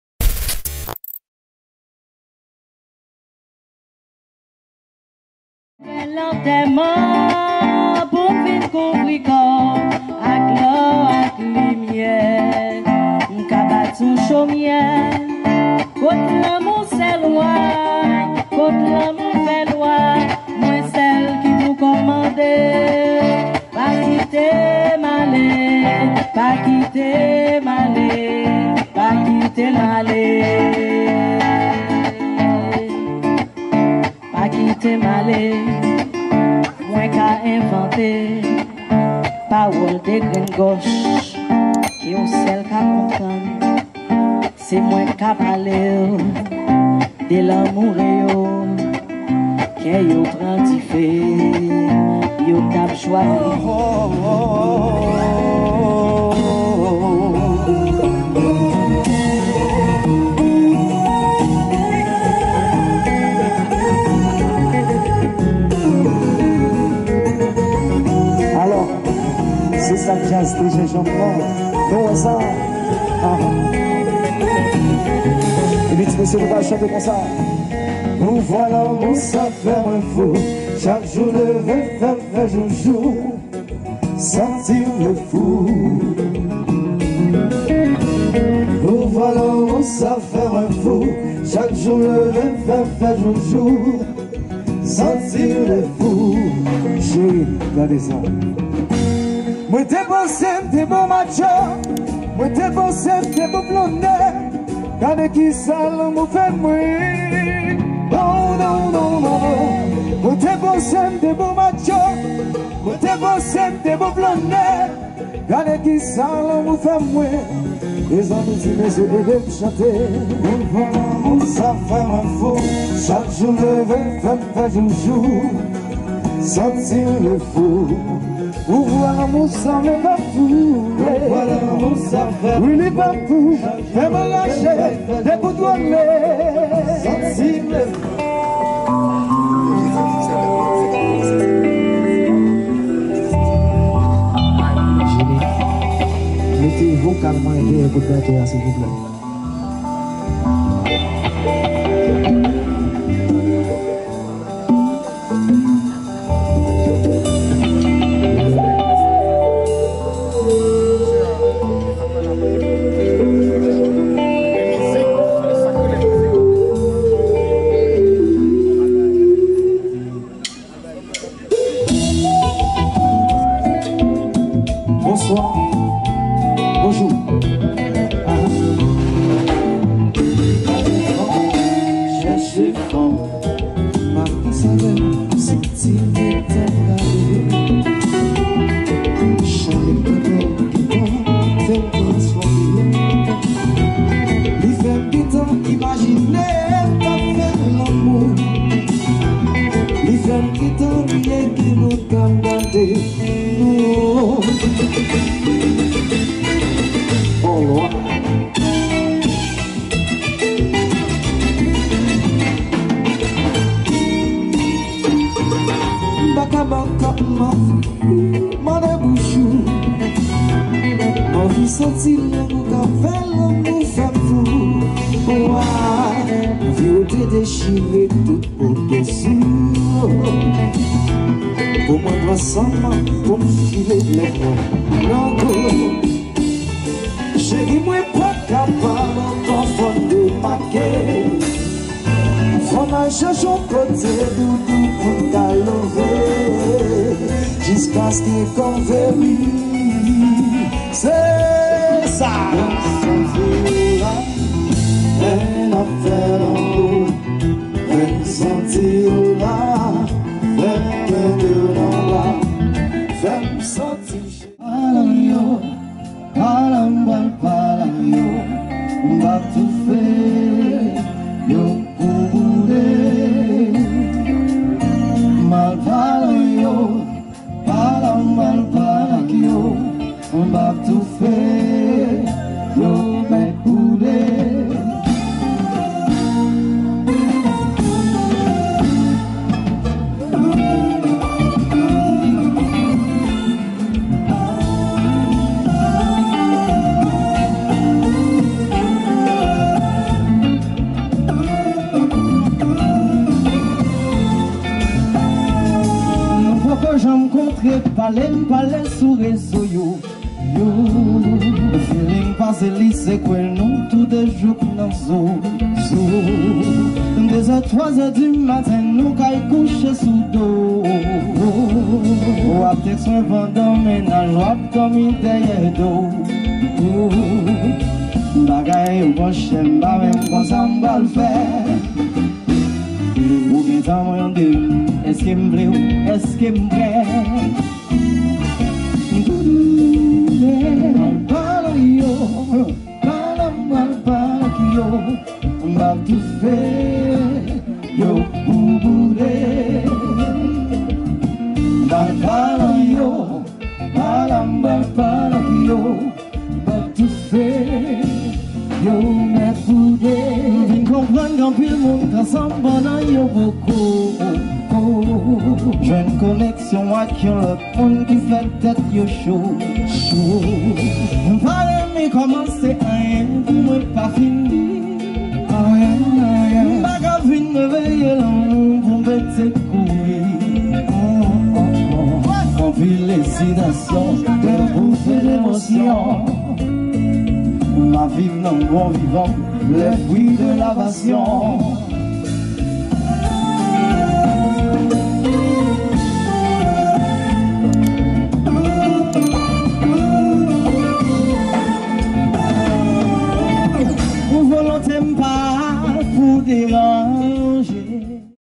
13ème Anniversaire G7 MIZIK Band (live)
13ème anniversaire G7 Band/ dimanche 26 juin 2022 à Vetiver hills